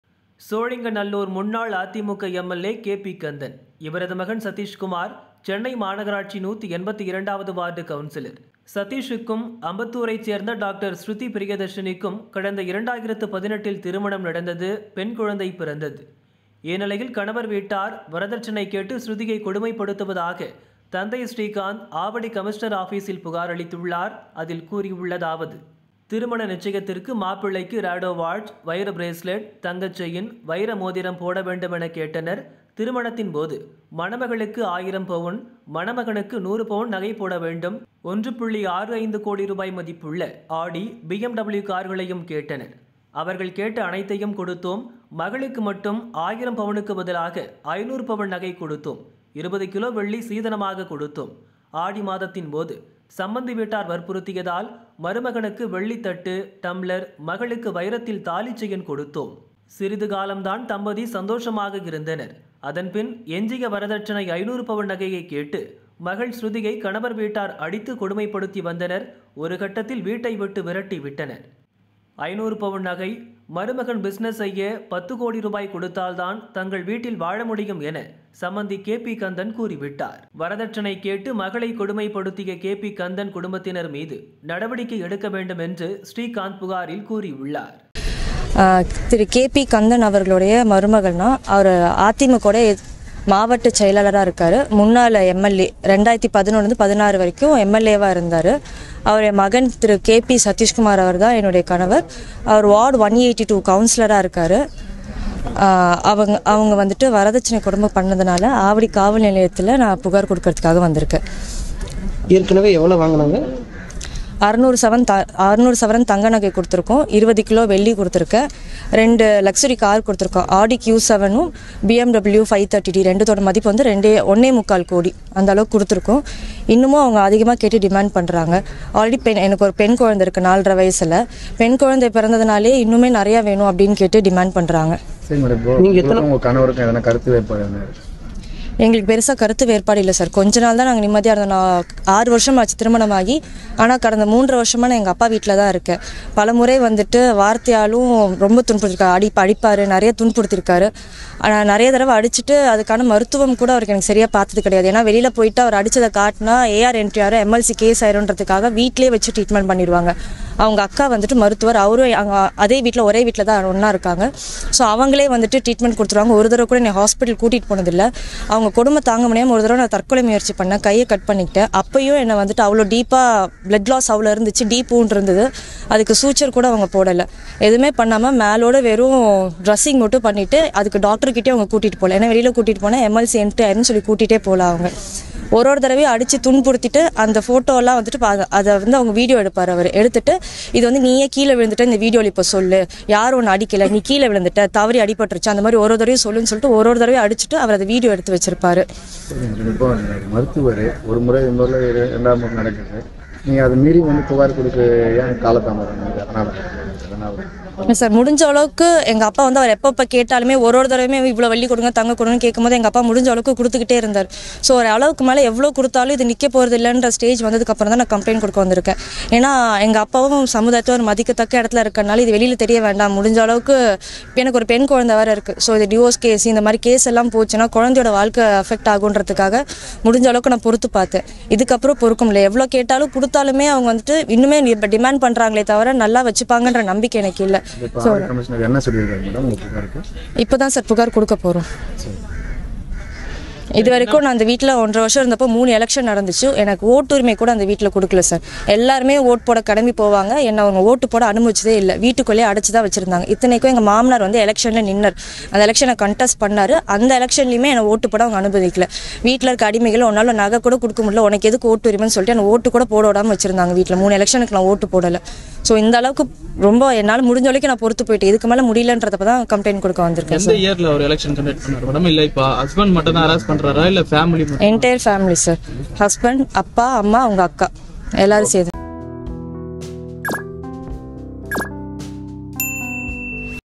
சென்னை மாநகராட்சி கவுன்சிலர் மனைவிக்கு நடந்த கொடுமை Ex MLA KP Kandhan| GCC councillor|dowry complai
மருமகளிடம் 500 பவுன் கேட்டு
துன்புறுத்திய மாஜி எம்எல்ஏ!

சென்னை மாநகராட்சி கவுன்சிலர் மனைவிக்கு நடந்த கொடுமை
Ex MLA KP Kandhan| GCC councillor|dowry complaint#